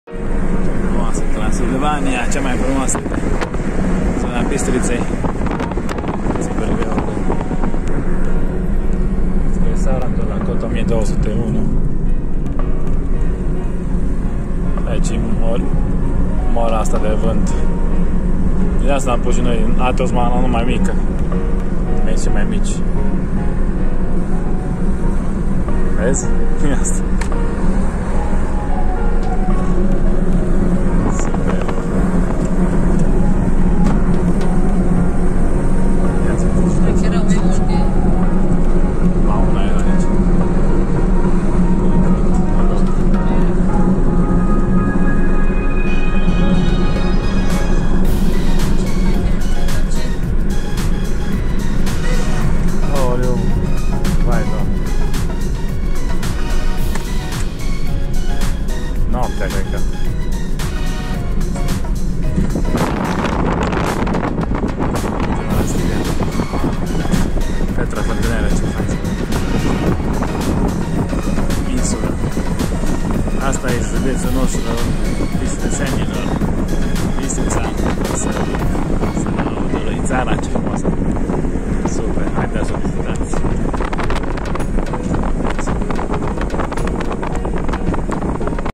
Pasul Tihuța din Transilvania ACASĂ Ultima Parte după Pelerinajul din Muntele Athos PA.31#
Finalul călătoriei mele a 3 din Muntele Athos se încheie aici. Bine eu am ajuns Acasă cu soremea Anastasia șoferița mea dragă 🤗🙏💙
A fost o Minune la întoarcere pe lângă că am venit Gratis și nam intrat în Carantină m-am întâlnit și cu Soremea la Fălticeni și ma luat de unde m-au lăsat Ucrainenii adică Bucovinenii 👼😎🇷🇴💙🙏🤗😁🥳🎉🥰 Slavă Lui Dumnezeu și Multumire Maicii Domnului ❤️💙💓